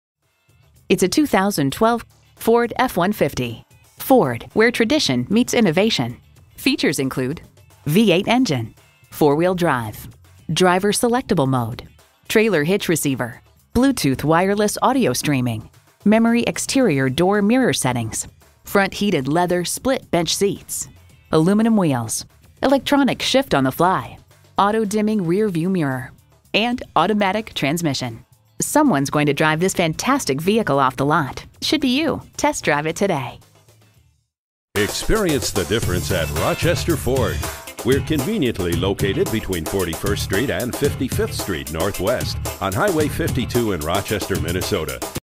Used 2012 Ford F-150 Rochester MN Winona, MN P11563 - SOLD
Year: 2012
Make: Ford
Model: F-150
Trim: Lariat
Engine: 3.5L V6
Transmission: 6-Speed Automatic Electronic
Color: Black
Interior: Pale Adobe
Mileage: 175185
Stock #: P11563
VIN: 1FTFW1ETXCFA13969
Meets our Rigorous Inspection and Reconditioning Standards! F-150 Lariat Tuxedo Black Metallic w/ Remote Start + Rear Camera CARFAX One-Owner. Clean CARFAX.

Address:
4900 Highway 52 North